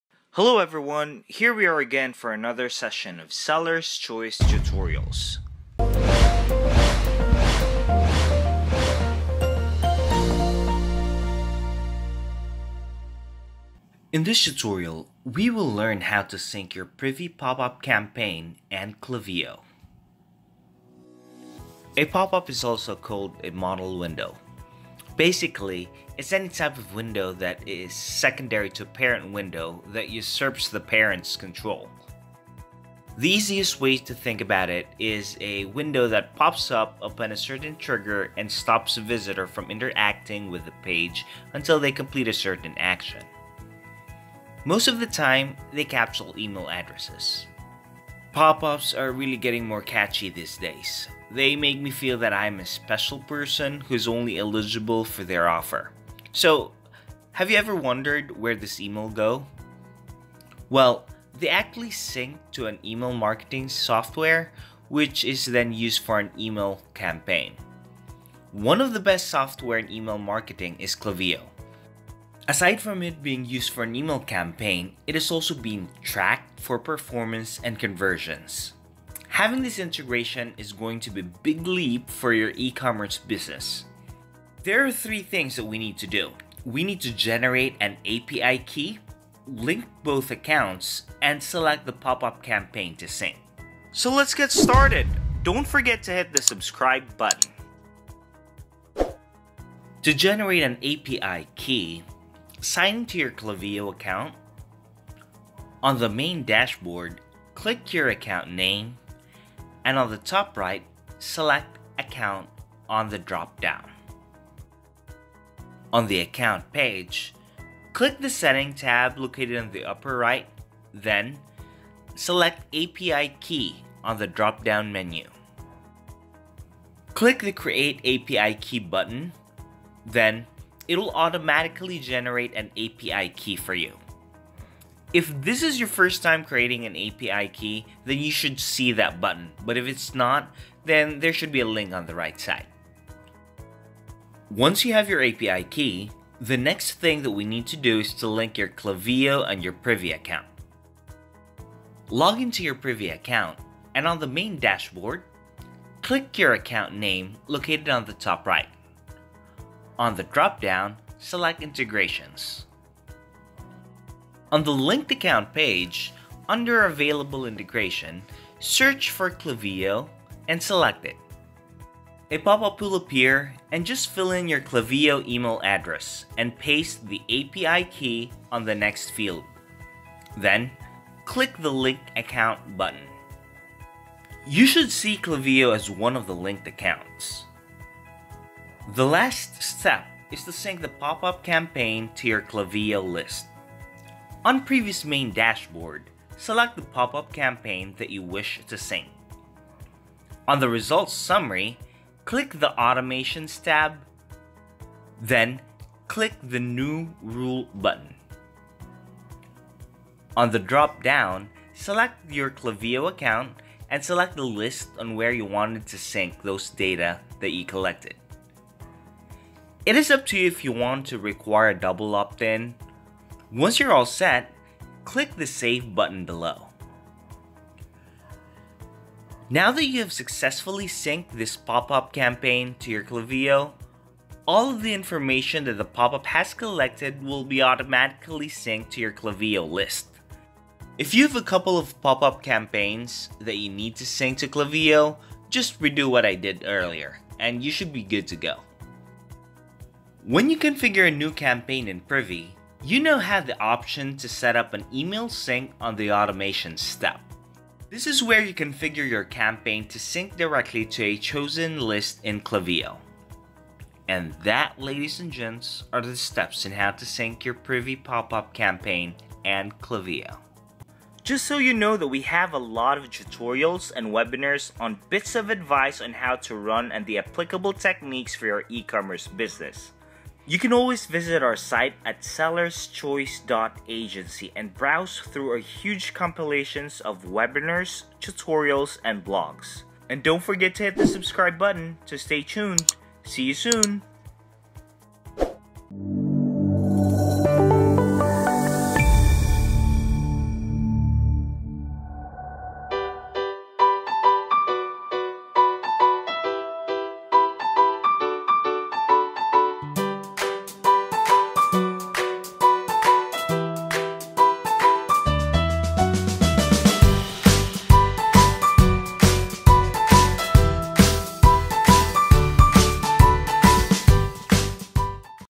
How to Sync your Privy Pop up Campaign and Klaviyo - E-commerce Tutorials
Today, we're showing you How to Sync your Privy Pop up Campaign and Klaviyo so you can sync all of your captured emails from privy and start an email campaign.